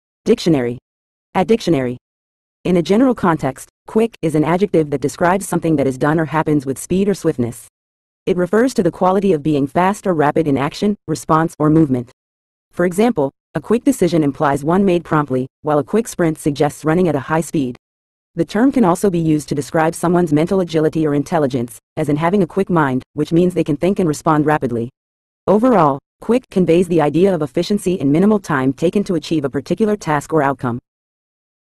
Quick Meaning In English
In a general context, "quick" is an adjective that describes something that is done or happens with speed or swiftness. It refers to the quality of being fast or rapid in action, response, or movement. For example, a quick decision implies one made promptly, while a quick sprint suggests running at a high speed. The term can also be used to describe someone's mental agility or intelligence, as in having a quick mind, which means they can think and respond rapidly. Overall, "quick" conveys the idea of efficiency and minimal time taken to achieve a particular task or outcome.